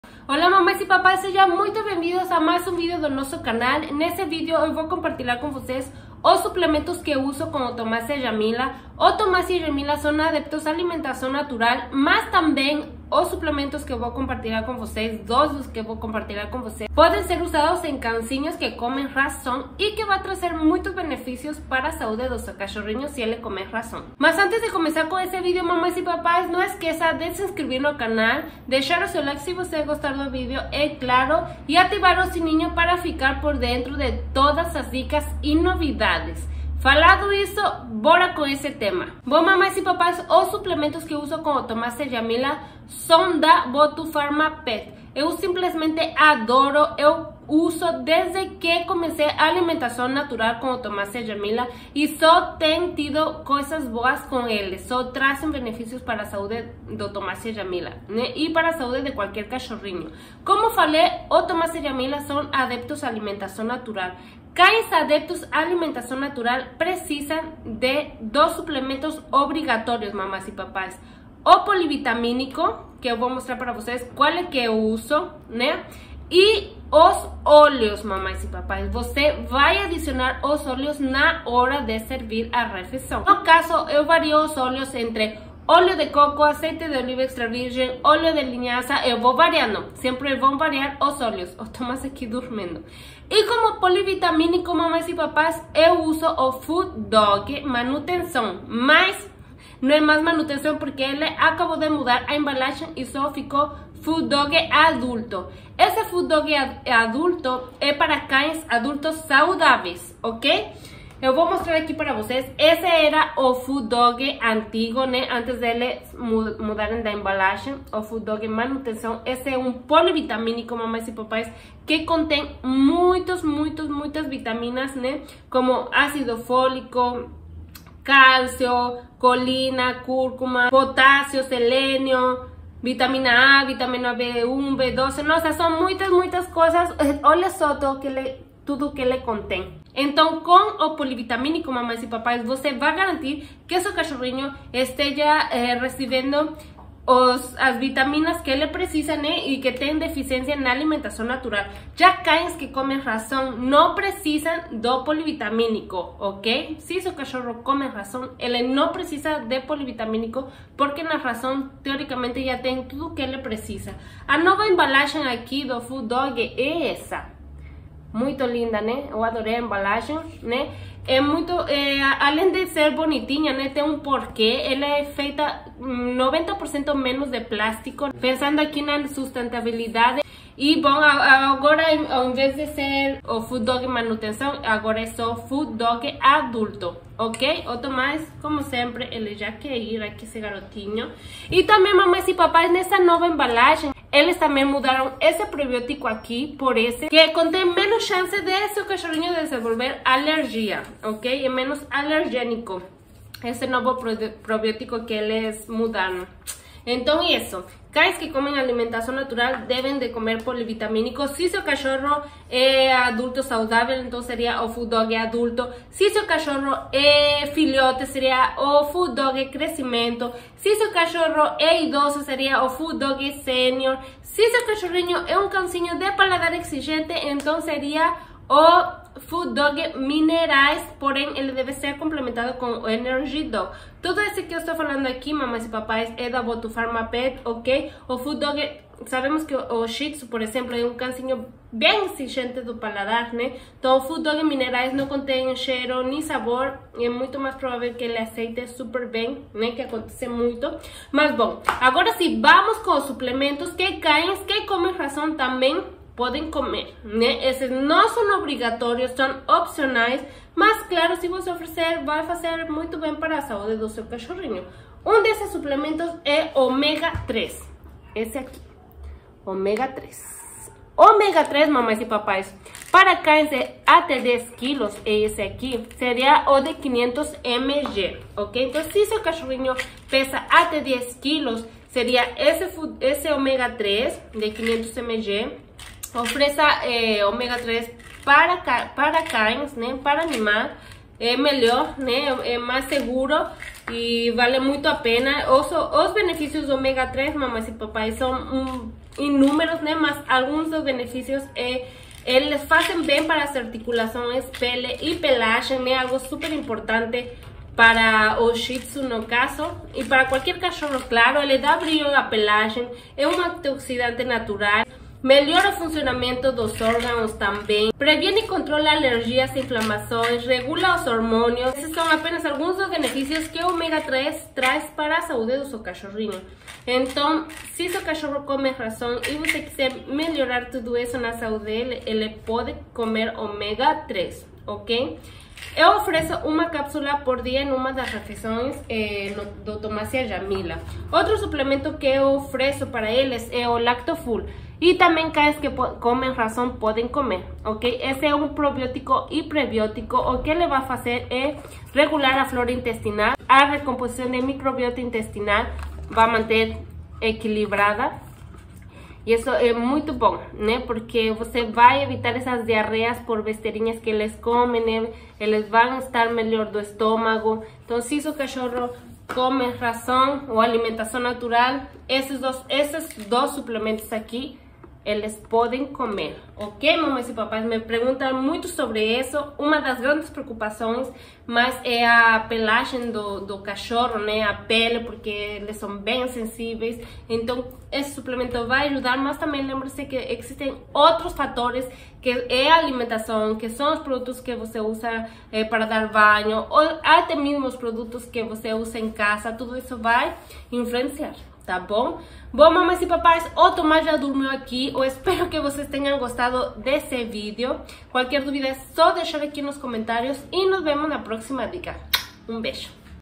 Suplementos para meus Shih Tzus | Alimentação natural e ração
Me perguntam muito sobre os suplementos que ofereço para o Thomas e a Jahmila, quais vitaminas usar para ajudar no crescimento dos pelos é uma das maiores dúvidas!

Como muitas de vocês sabem  o Thomas e a Jahmila são adeptos a alimentação natural há 6 anos e cães que comem comida natural devem comer suplementos (obrigatórios) para que a alimentação esteja balanceada.

Nesse vídeo compartilho com vocês quais suplementos obrigatórios ofereço para eles e quais opcionais que cães que comem ração,  e que vai trazer muitos benefícios para a saúde do seu cachorrinho

Suplementos: @botupharmapet @botupharma

Se você tem vontade de aprender sobre alimentação natural cozida clique no link e veja como o Curso Alimentação Natural Mundo Shih Tzu pode te ajudar